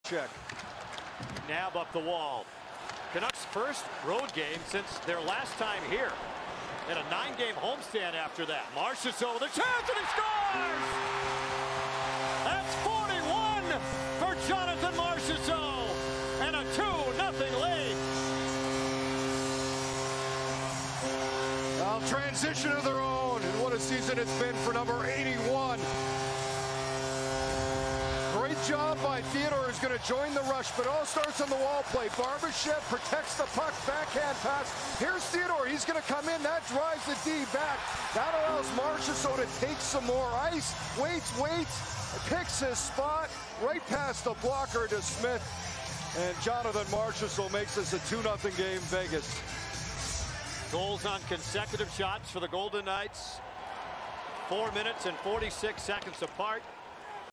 Ivan Barbashev assists on Marchessault's goal vs Canucks (2 apr 2024)
Иван Барбашев помог Маршессо забить "Ванкуверу".